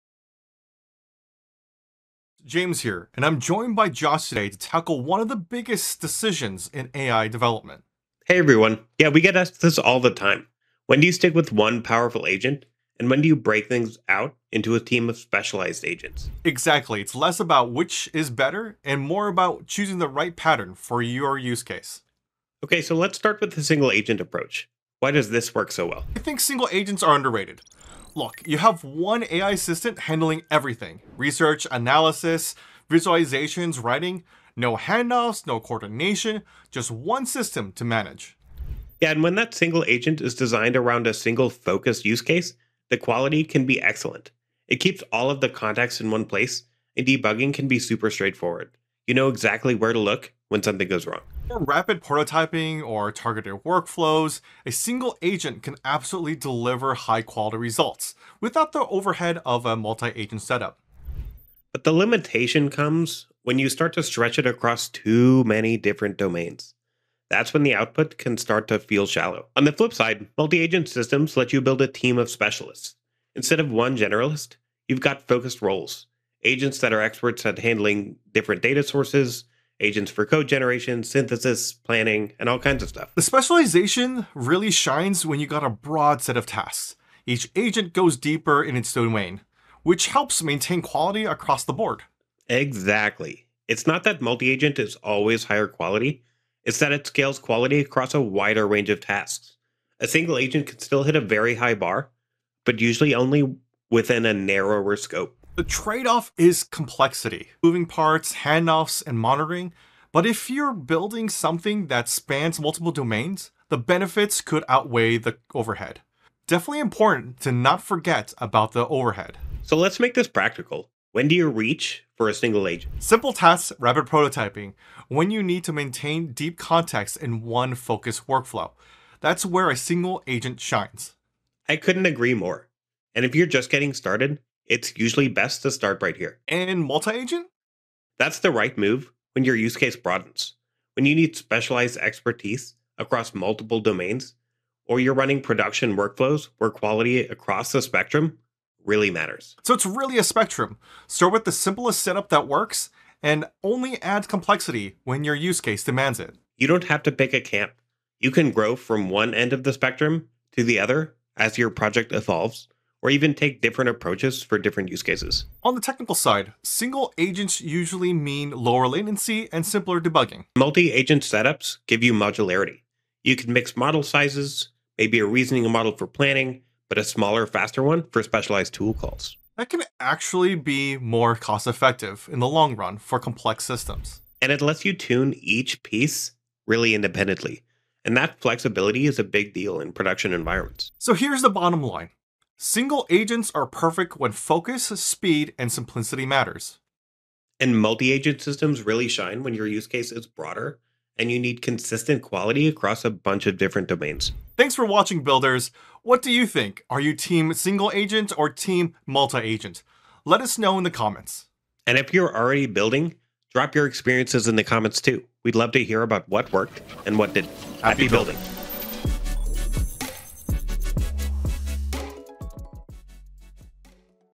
Single Agent Vs. Multi-Agent Systems in AI | How To Choose The Right Architecture
When building with AI, one of the biggest decisions developers face is choosing between a single-agent or a multi-agent system. In this video, we explore which AI architecture is right for your project — and how to get the best performance from your models.

You’ll learn:
- What single-agent AI systems are best for
- When to scale into multi-agent AI architectures
- How tools like LangGraph and CrewAI make orchestration easier
- The trade-offs in coordination, debugging, and scalability
- Why Snowflake Cortex can power agent-based development.

Watch if you’re building with AI, exploring agent orchestration, or want to improve your system design.

Click here to start your 30-day free Snowflake trial, which includes $400 worth of free usage: